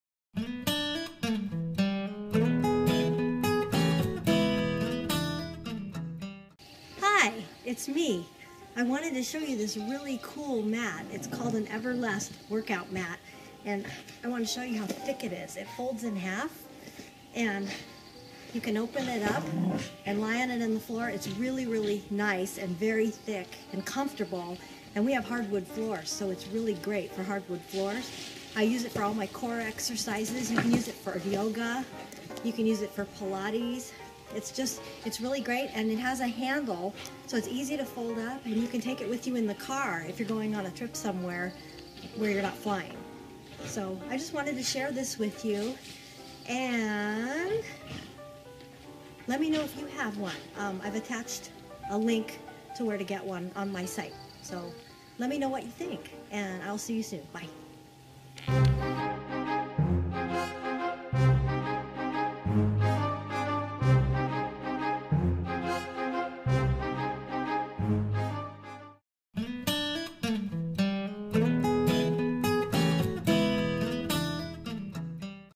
Using An Exercise Mat Is Great For Developing Core Strength!
Do you do stretching exercises, Pilates, yoga or meditation?

Health and Fitness is important! What I discovered through the years is that stretching should be incorporated into workout routines, no matter how boring it is!  I wish I would've learned this earlier in life!

Great For Lower Back Exercises!

Many people use a yoga mat to relax on. Yoga mats are very convenient, but I have a hardwood floor and need something a little thicker especially when I’m performing core exercises!

It Folds In Half!

Not only is the Everlast workout mat thick enough to protect your body, it also folds in half and has a handle, which makes for easy storage. Using this mat has been instrumental for my recovery from injuries.

Balance On One Foot!

Besides stretching on it, I also use it for cushion when doing lunges with the Functional trainer as well as standing on it, balancing on one foot. I bet you’d like to see that, huh? LOL